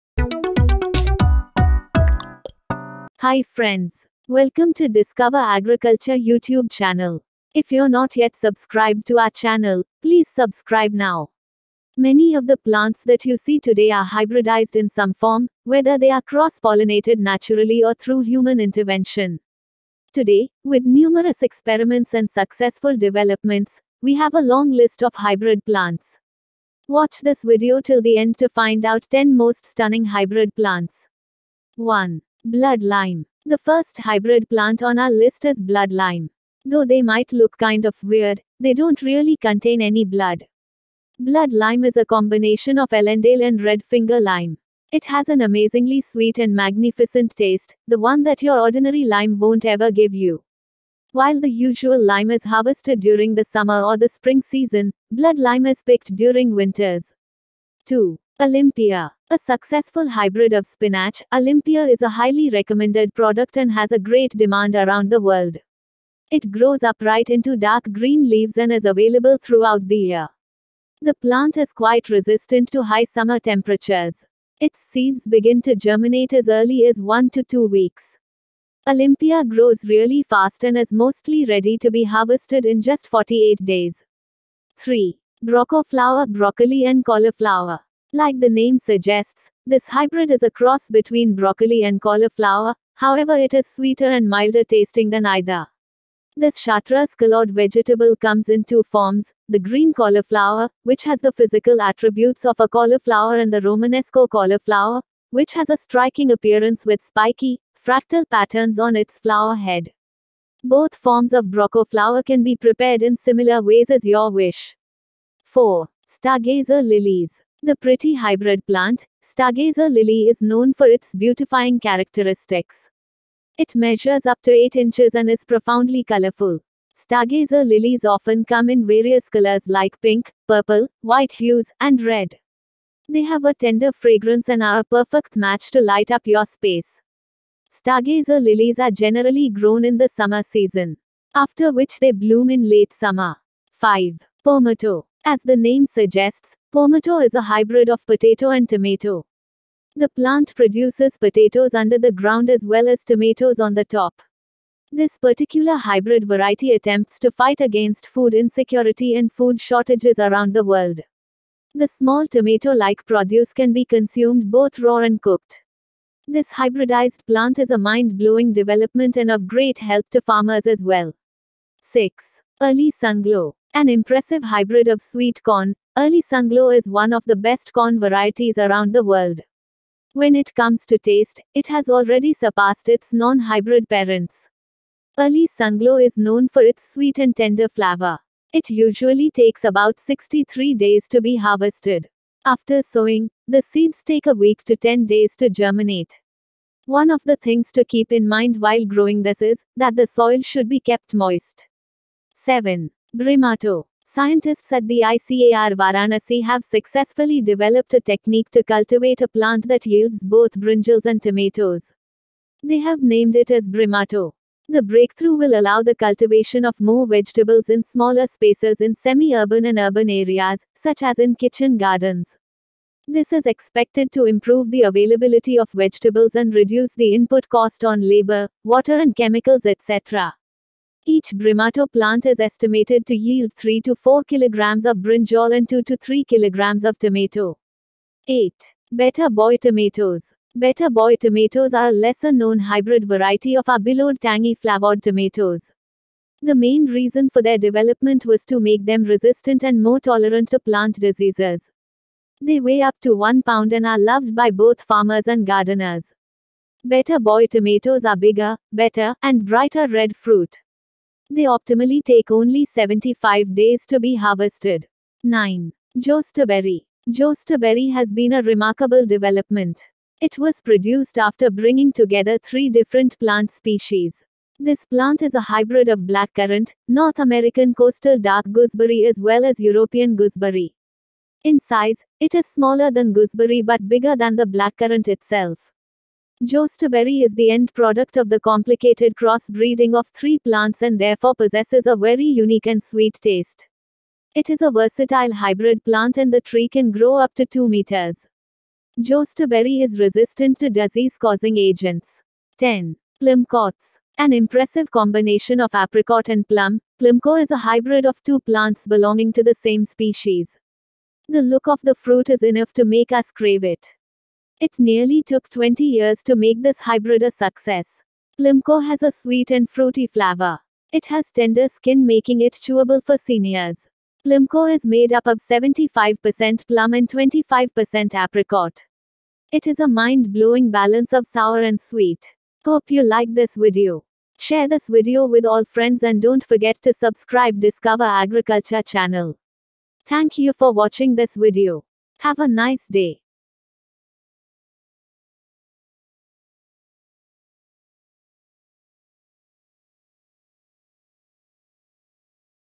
Strange Hybrid Plants that Actually Exist
Many of the plants that you see today are hybridized in some form, whether they are cross-pollinated naturally or through human intervention. Today, with numerous experiments and successful developments, we have a long list of hybrid plants. Watch this video till the end to find out the 10 most stunning hybrid plants:

0:00 Introduction
0:11 Strange Hybrid Plants that Actually Exist
0:31 Blood Lime
0:57 Olympia
1:24 Broccoflower
1:55 Stargazer Lilies
2:22 Pomato
2:51 Early Sunglow
3:21 Brimato
3:57 Better Boy Tomatoes
4:25 Jostaberry
5:02 Plumcots
5:32 Bottom line